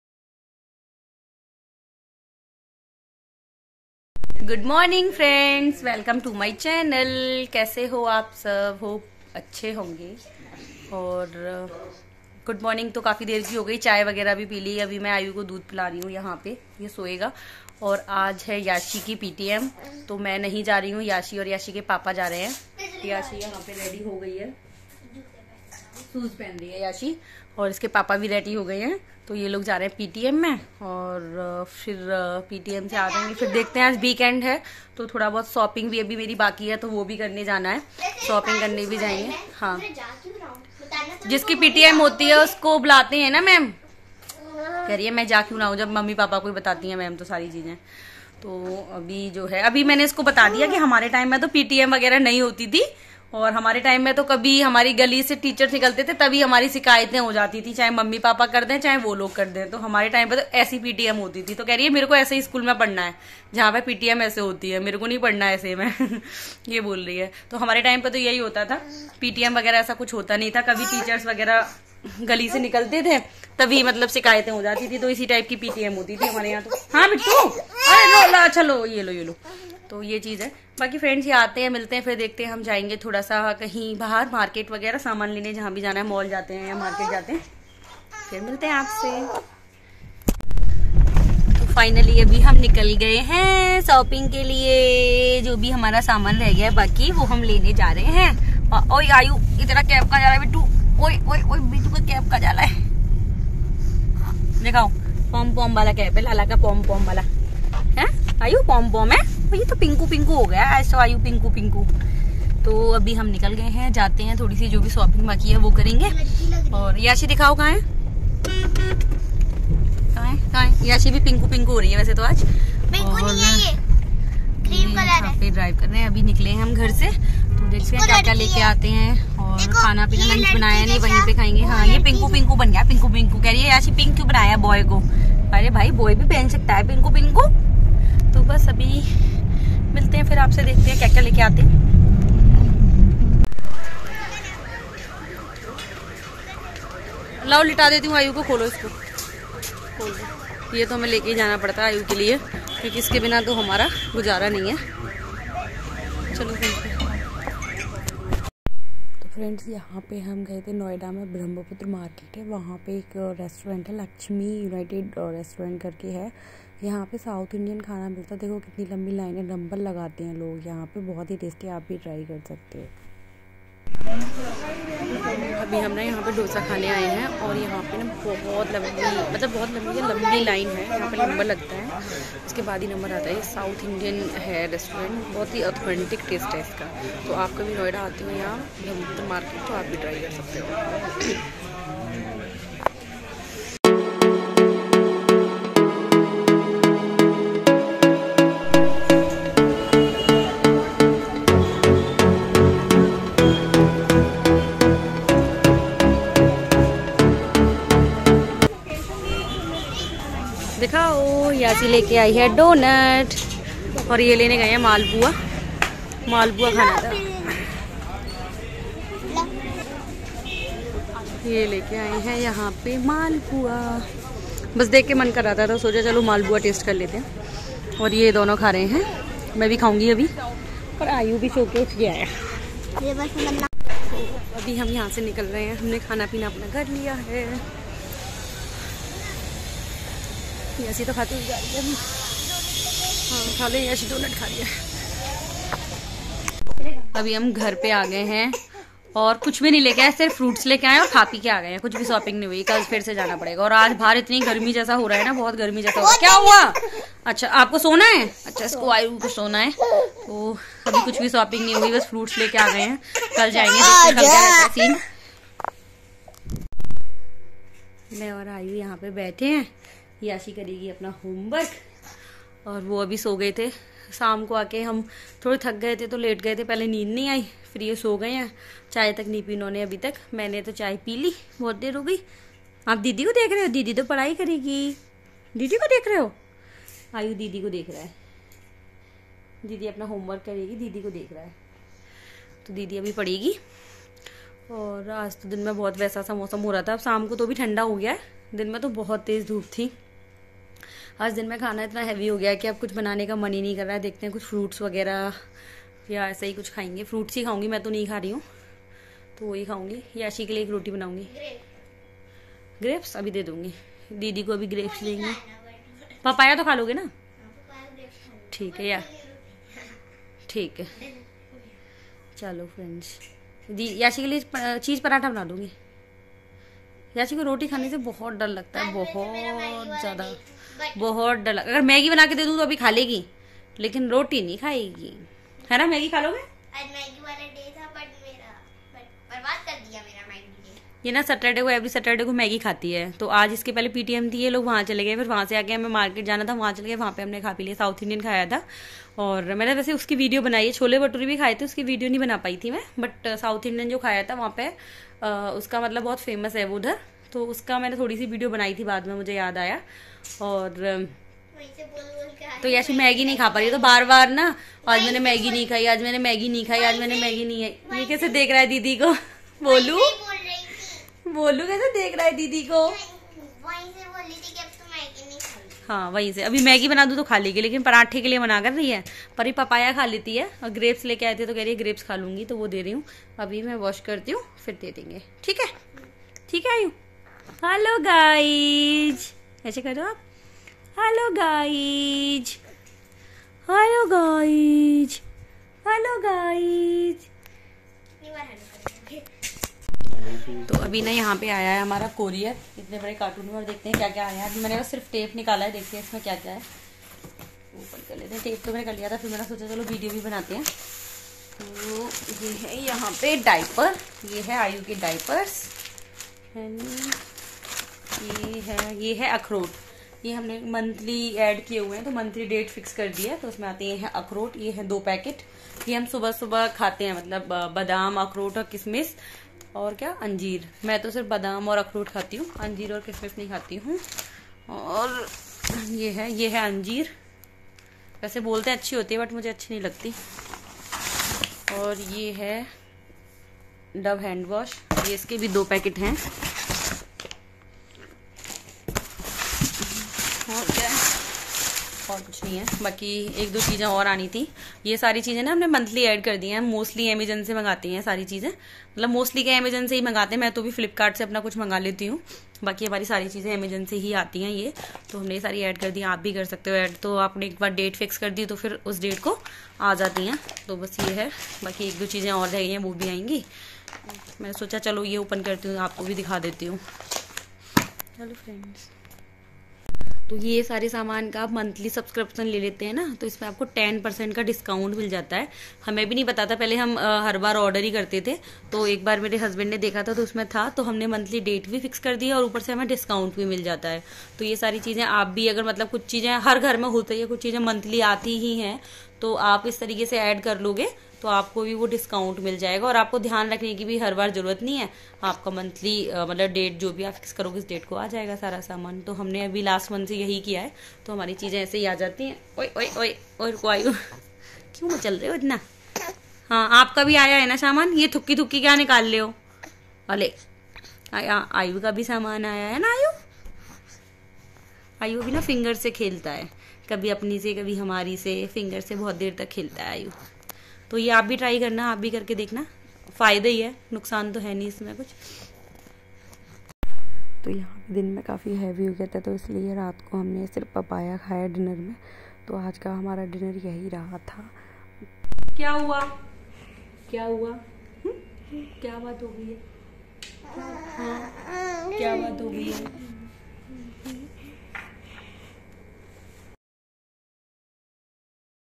Famous South Indian Restaurant Lakshmi Coffee House (Brahmaputra Market) Gaye Aaj | Yummy Food
If you are also a mumma, you will be able to connect surely with mother's life.
Hit likes button.